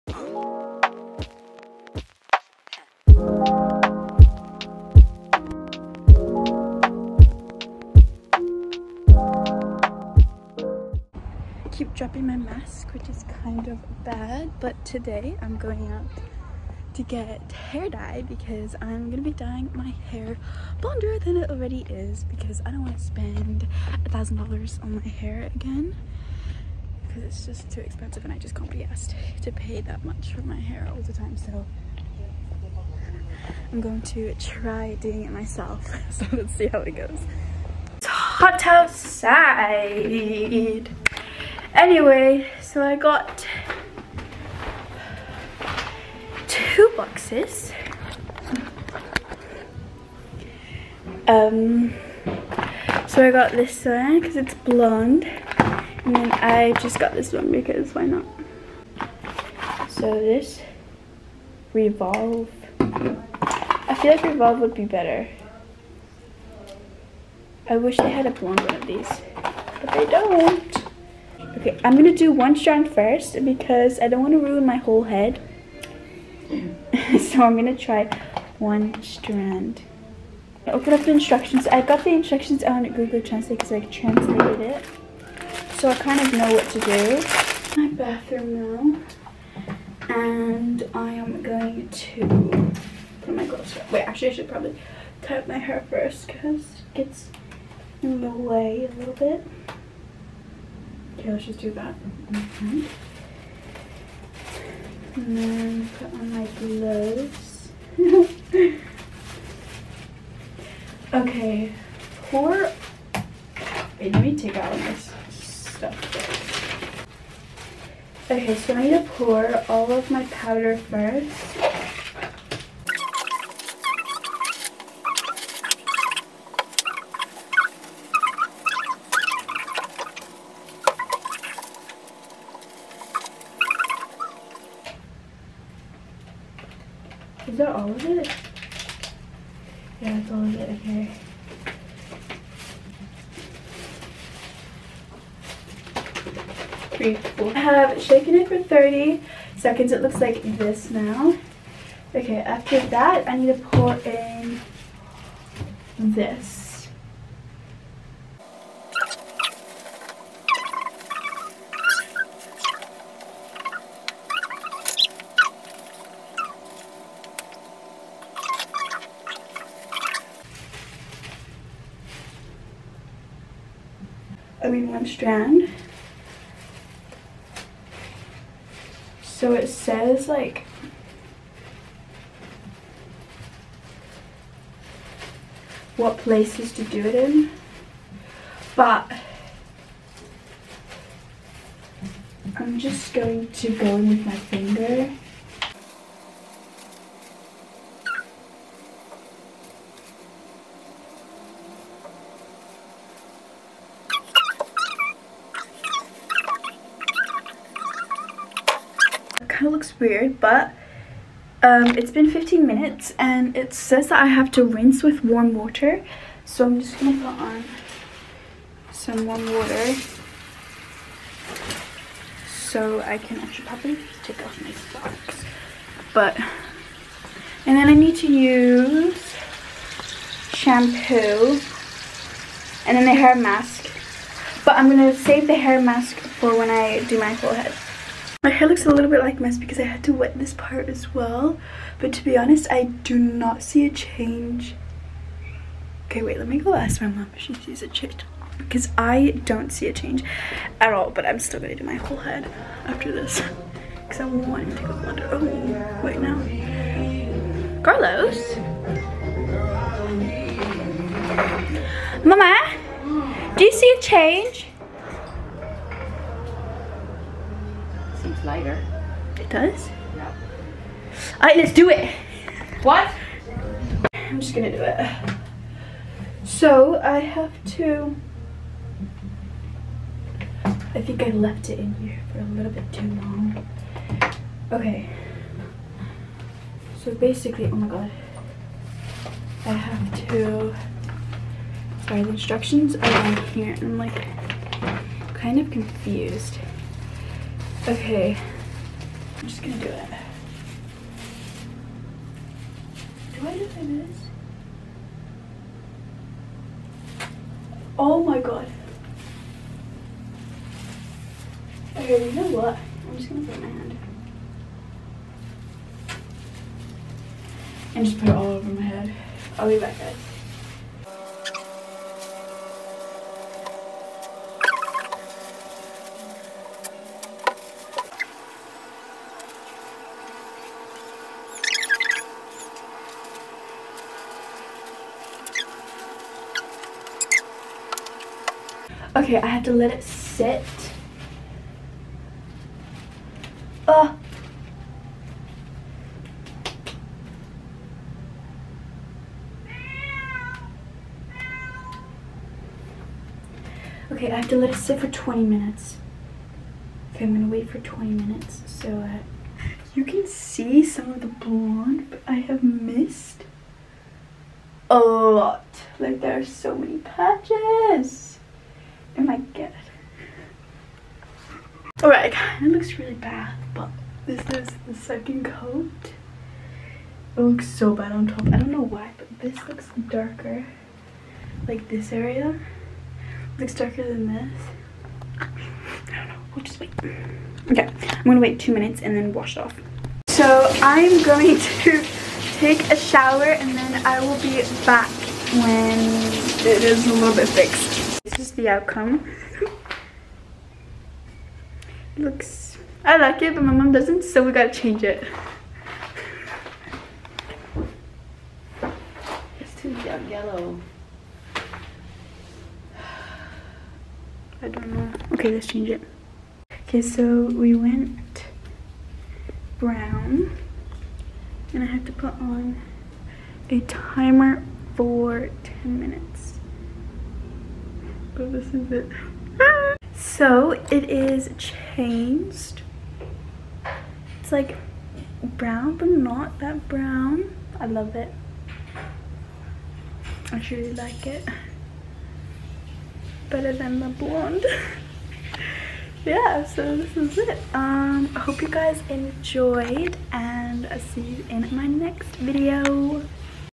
I tried dyeing my hair and it went COMPLETELY WRONG!
xx